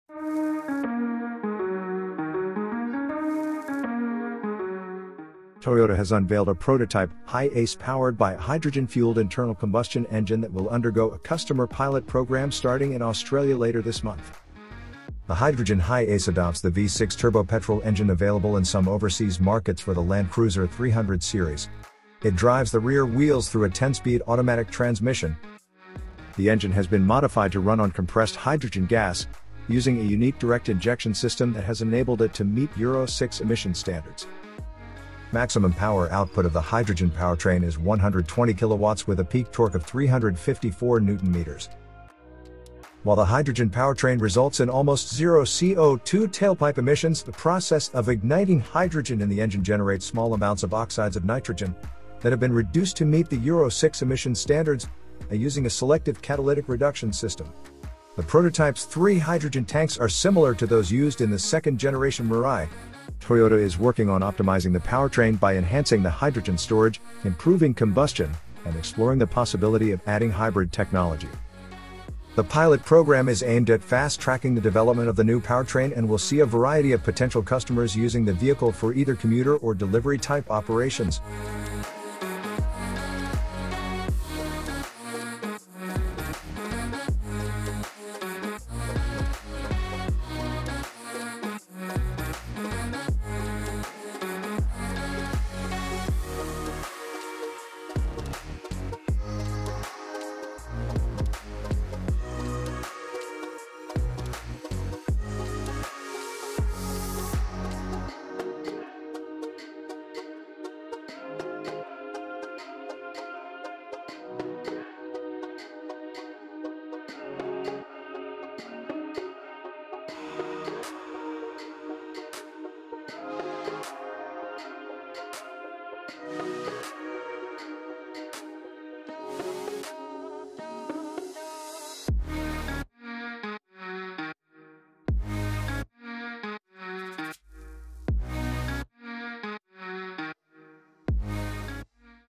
New Toyota HYDROGEN HiAce Prototype With V6 Turbo Engine
Toyota has introduced a prototype HiAce equipped with a hydrogen-fueled internal combustion engine, marking a significant step in the company's global approach to decarbonization. The hydrogen-powered HiAce, set to undergo a customer pilot program in Australia, is part of Toyota's commitment to various pathways for reducing carbon emissions, including investments in carbon-neutral fuels, hybrid-electric, plug-in hybrid-electric, battery-electric, and fuel-cell electric vehicles. The Australian pilot program aims to evaluate the technology in real-world conditions, with selected companies testing the fit-for-purpose commercial vehicle's ultra-low CO2 tailpipe emissions. Toyota Australia President and CEO Matthew Callachor expressed pride in operating this pilot program, emphasizing the potential for hydrogen-fueled internal combustion engines to meet commercial customers' practical needs while significantly reducing CO2 emissions.

The hydrogen-powered HiAce features a V6 turbo petrol engine modified to run on compressed hydrogen gas, meeting Euro VI emission standards. The prototype's three hydrogen tanks are similar to those used in the second-generation Mirai, and Toyota plans to optimize the powertrain by improving hydrogen storage, combustion, and exploring hybrid technology. The pilot program, involving companies such as CPB Contractors for commuter and delivery-type operations, aims to fast-track the powertrain's development. Toyota's commitment to Australia's hydrogen economy includes previous trials with Mirai FCEV, the establishment of a hydrogen production facility, and a Memorandum of Understanding with Hyundai Australia, Ampol, and Pacific Energy for hydrogen refueling infrastructure development. Additionally, Toyota plans to assemble fuel-cell generators at its Altona facility for distribution in Australia and New Zealand.

Source : Toyota